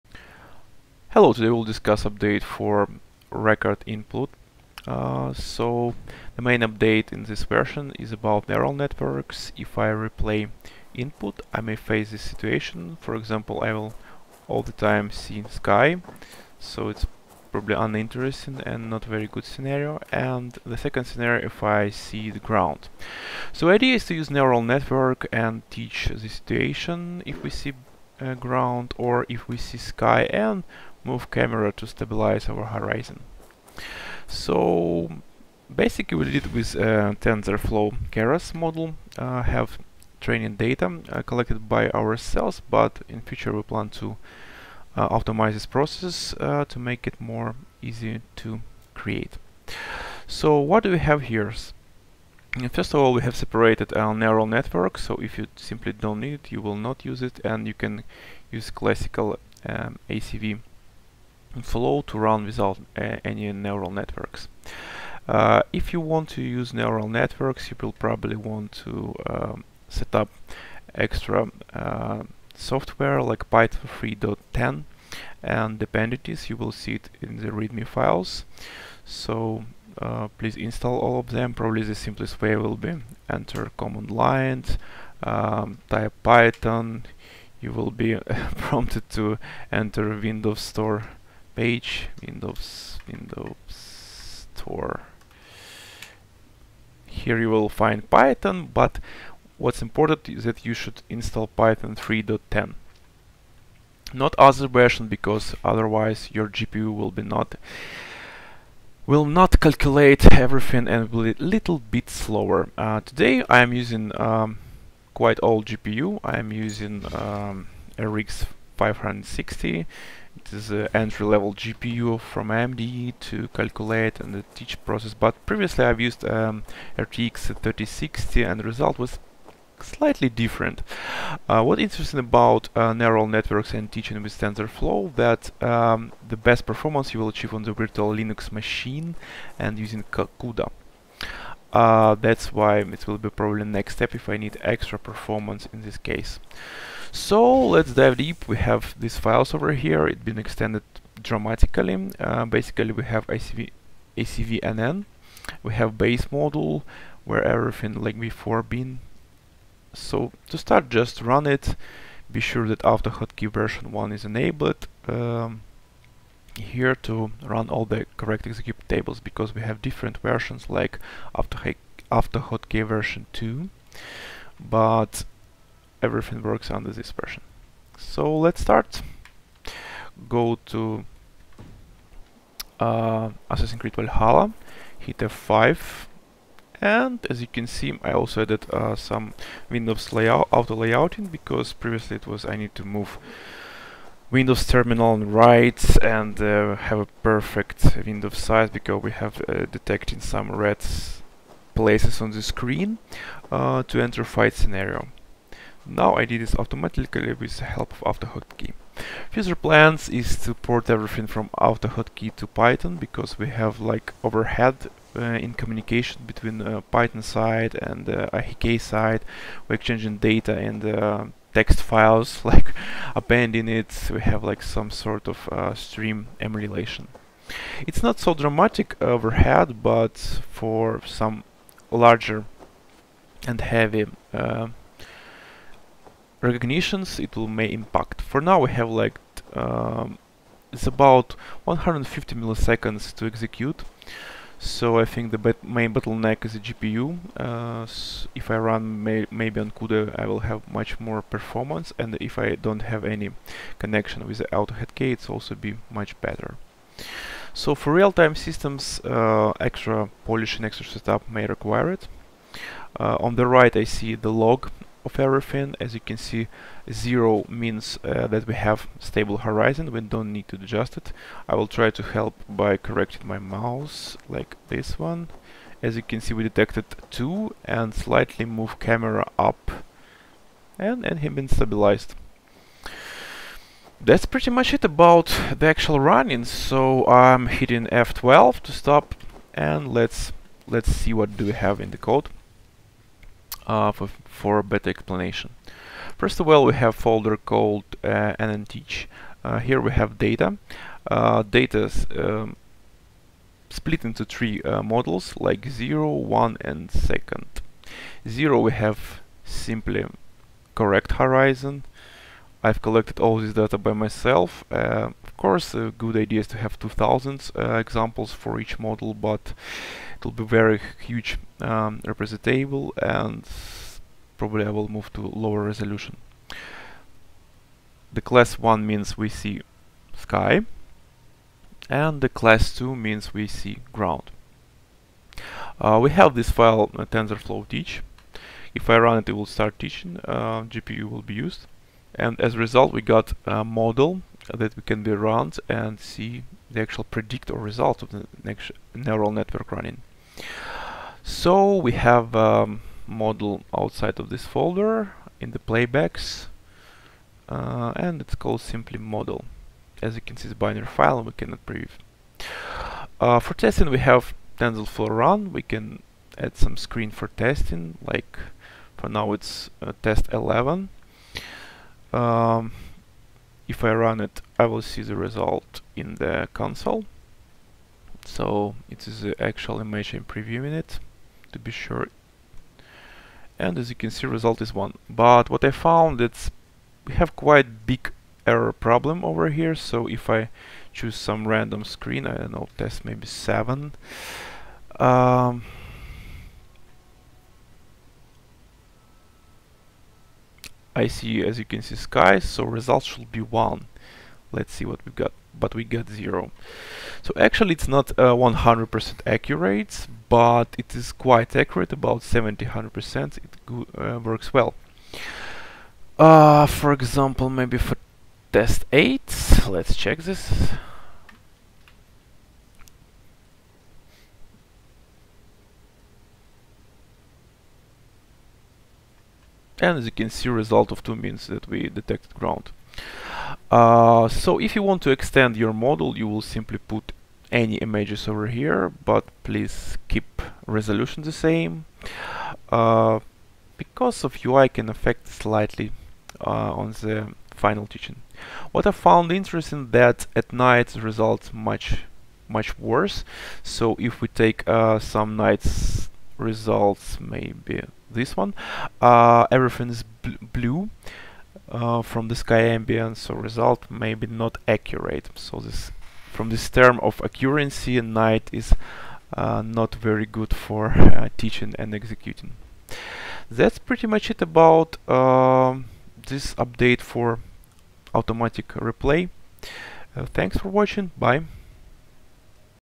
Game Input Recorder Neural Networks Update 2023.10
- Added Tensorflow Keras image classification for stabilize camera horizon
- Added windows auto layout for easier workflow
- Separate classic record and neural networks